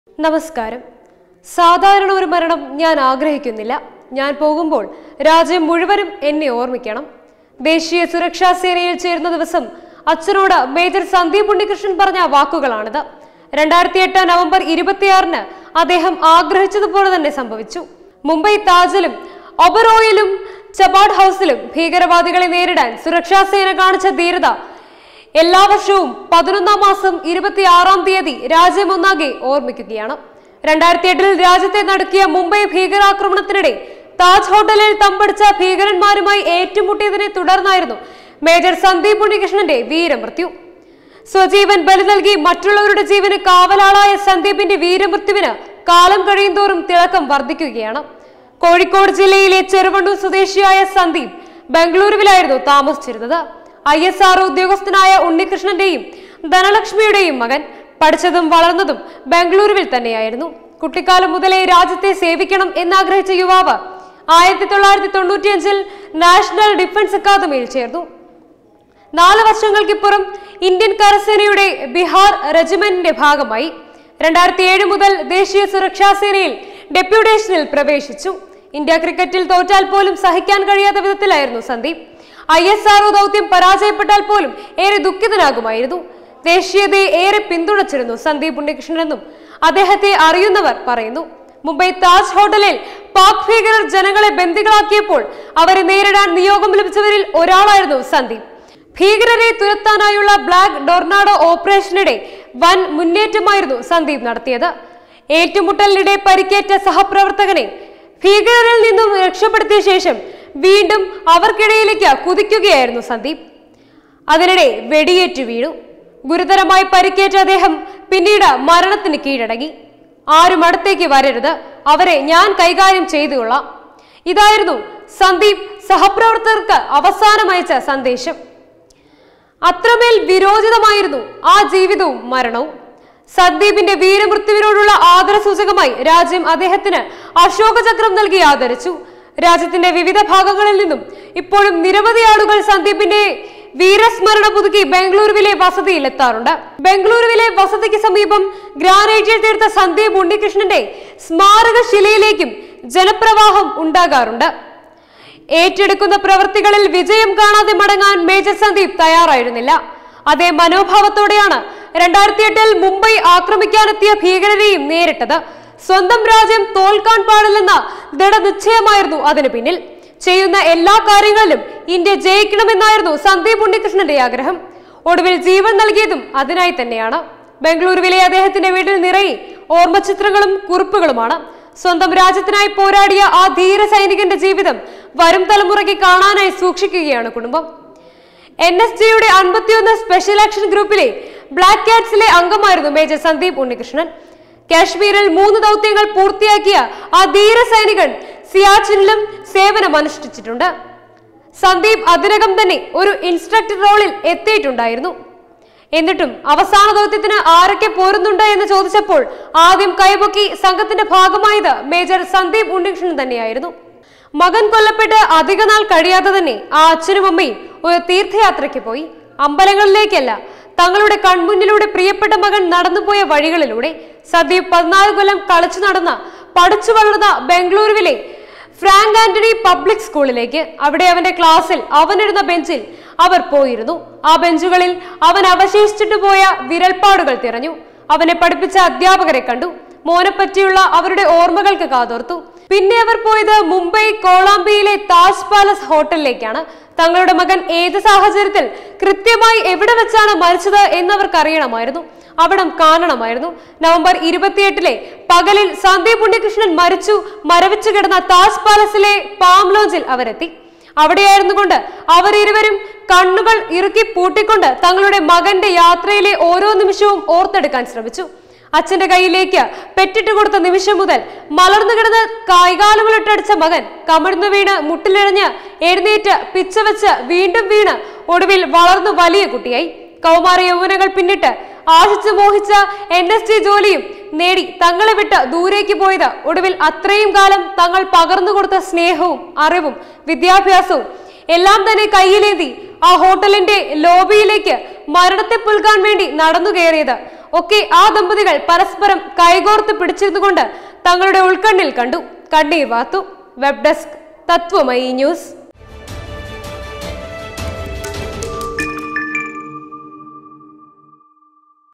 ഞാൻ പോകുമ്പോൾ രാജ്യം മുഴുവനും എന്നെ ഓർമിക്കണം ഇത് രാജ്യം എന്നും ഓർമ്മിക്കുന്ന ധീരന്റെ വാക്കുകൾ
ഞാൻ പോകുമ്പോൾ രാജ്യം മുഴുവനും എന്നെ ഓർമിക്കണം ഇത് രാജ്യം എന്നും ഓർമ്മിക്കുന്ന ധീരന്റെ വാക്കുകൾ | SANDEEP UNNIKRISHNAN